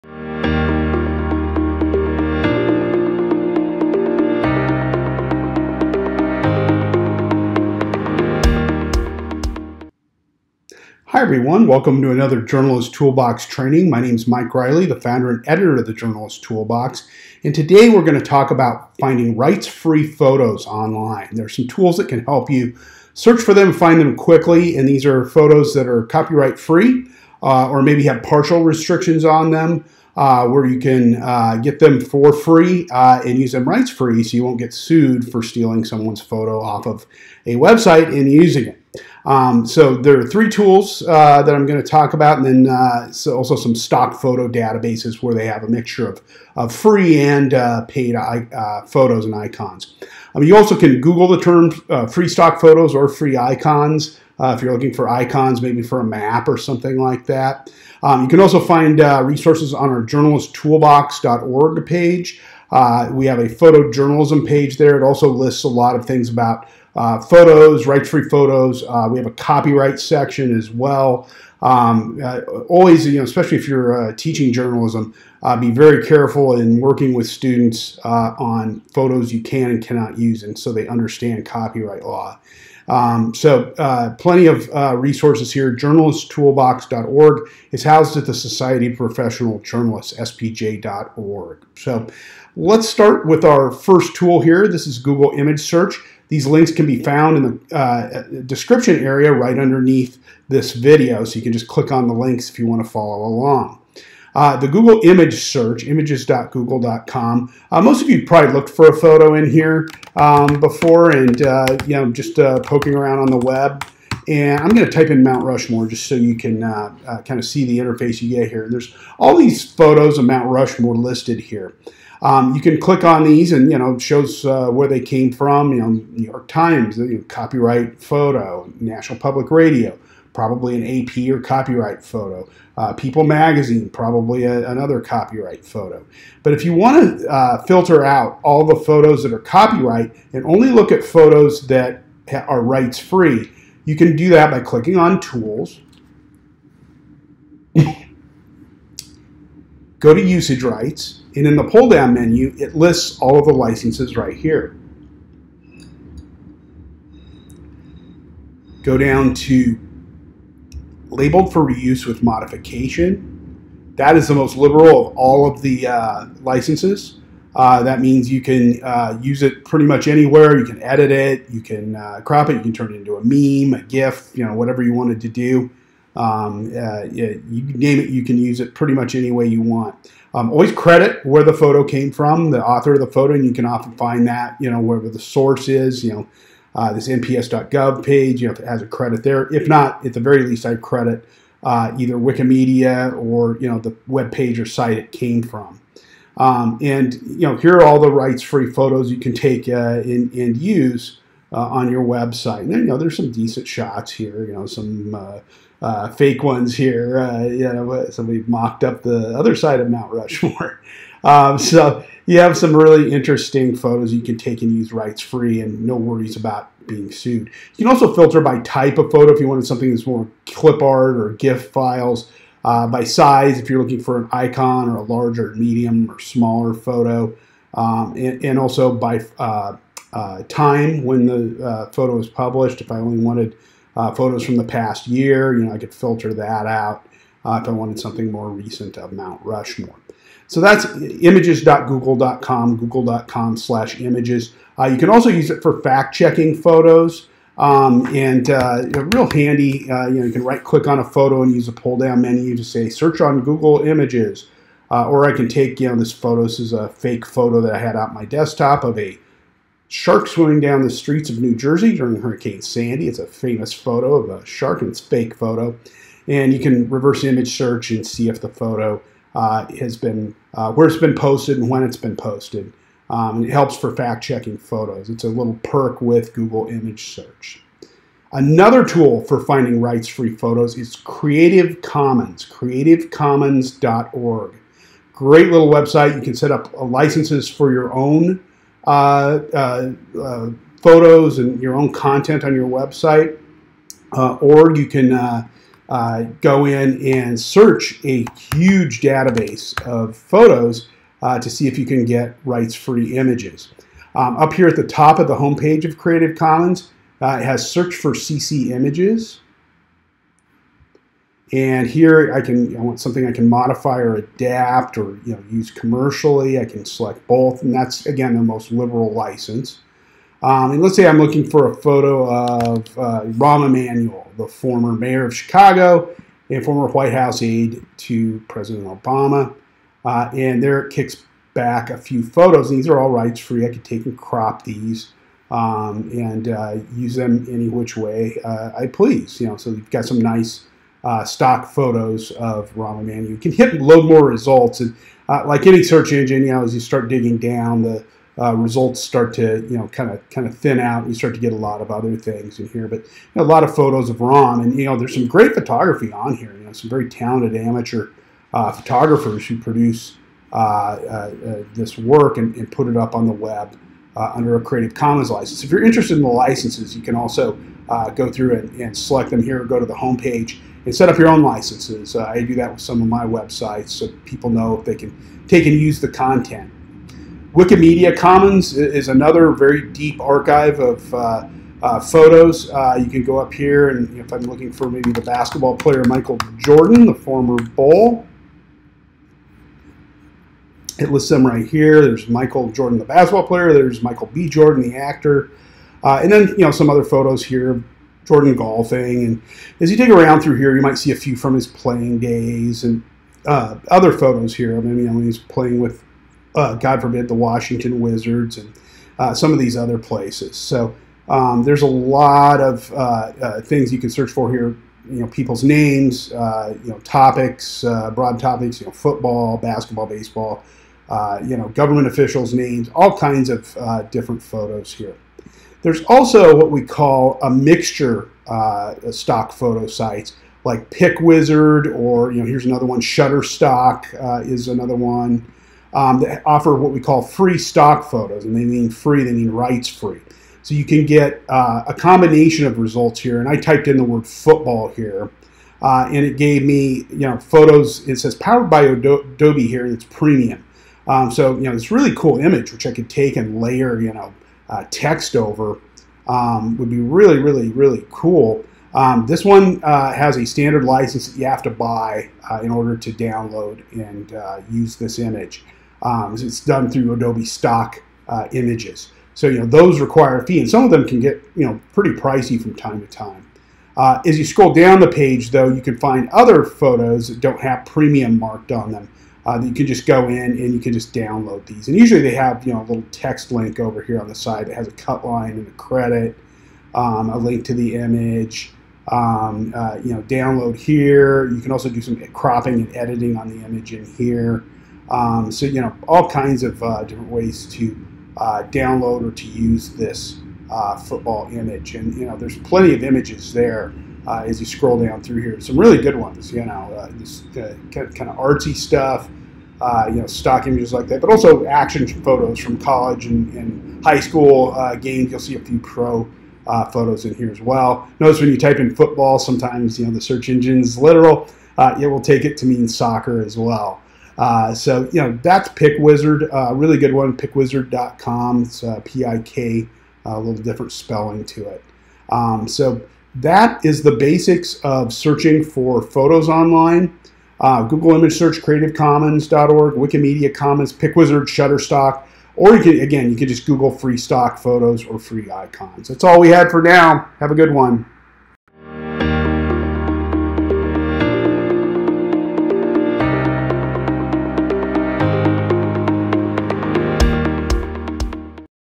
Journalist's Toolbox: Find Rights-Free Photos
In this episode, Mike Reilley shows you how to locate rights-free photos on the web:

Stock Photo Databases

or Google “free stock photos” or “free icons”

** Music rights-free via Fiverr